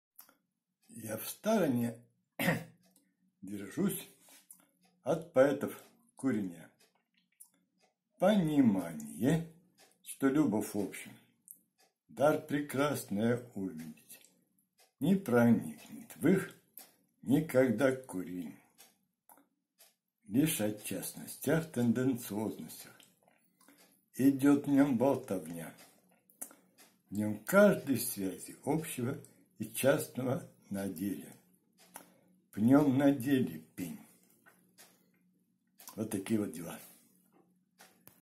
В стороне держусь от поэтов куреня
Я в стороне держусь  от поэтов куреня.
Понимание, что любовь, в общем, — дар прекрасное увидеть, не проникнет в их никогда курень.
Лишь о частностях-тенденциозностях идет в нем болтовня.
В нем каждый в связи общего и частного на деле пнем на деле пень.